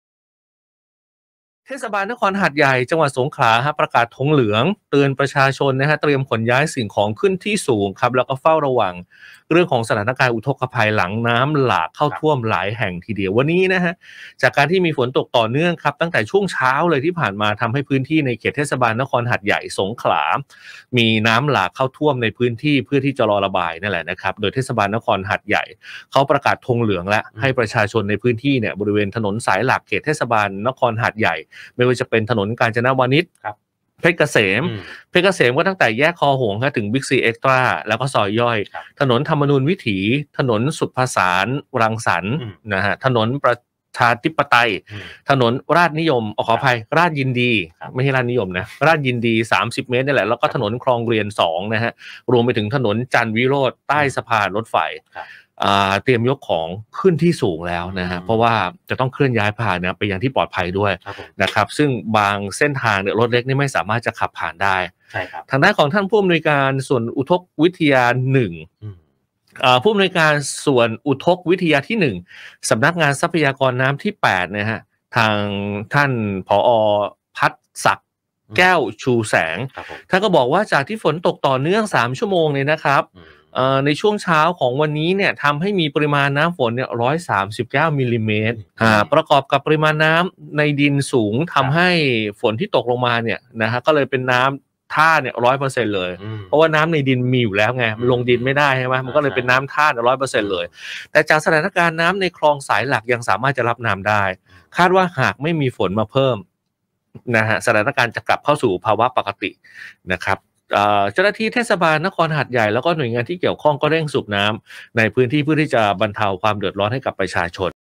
หาดใหญ่ ประกาศธงเหลืองเฝ้าระวังสถานการณ์อุทกภัย
เทศบาลนครหาดใหญ่ จ.สงขลา ประกาศธงเหลือง เตือนประชาชนเตรียมขนย้ายสิ่งของขึ้นที่สูง เฝ้าระวังสถานการณ์อุทกภัย หลังน้ำหลากเข้าท่วมหลายแห่ง‼️‼️

วันนี้ (21 ธ.ค.65) จากการที่มีฝนตกต่อเนื่องตั้งแต่ช่วงเช้าที่ผ่านมาทำให้พื้นที่ในเขตเทศบาลนครหาดใหญ่ จ.สงขลา มีน้ำหลากเข้าท่วมในพื้นที่เพื่อรอการระบาย โดยเทศบาลนครหาดใหญ่ ประกาศธงเหลือง ให้ประชาชนในพื้นที่ บริเวณถนนสายหลักเขตเทศบาลนครหาดใหญ่ ได้แก่ ถนนกาญจนวนิช ถนนเพชรเกษม (ตั้งแต่แยกคอหงส์ - บิ๊กซีเอ๊กซ์ตร้าและซอยย่อย) ถนนธรรมนูญวิถี ถนนศุภสารรังสรรค์ ถนนประชาธิปัตย์ ถนนราษฎร์ยินดี (30 เมตร) ถนนคลองเรียน 2 และถนนจันทร์วิโรจน์ (ใต้สะพานรถไฟ) เตรียมยกของขึ้นที่สูงและเคลื่อนย้ายยานพาหนะไปยังที่ปลอดภัย ซึ่งบางเส้นทางรถเล็กไม่สามารถขับผ่านได้

ทางด้านนายพัฒศักดิ์ แก้วชูแสง ผู้อำนวยการส่วนอุทกวิทยาที่ 1 สำนักงานทรัพยากรน้ำที่ 8 เปิดเผยว่า จากฝนที่ตกต่อเนื่อง 3 ชั่วโมง ในช่วงเช้าของวันนี้ ทำให้มีปริมาณน้ำฝน 139 มม. ประกอบกับปริมาณน้ำในดินสูงทำให้ฝนที่ตกลงมาเป็นน้ำท่า 100% แต่จากสถานการณ์น้ำในคลองสายหลักยังสามารถรับน้ำได้ คาดว่าหากไม่มีฝนตกเพิ่มสถานการณ์จะกลับสู่สภาวะปกติ

เจ้าหน้าที่เทศบาลนครหาดใหญ่และหน่วยงานที่เกี่ยวข้อง เร่งสูบน้ำในพื้นที่ เพื่อบรรเทาความเดือดร้อนให้ประชาชน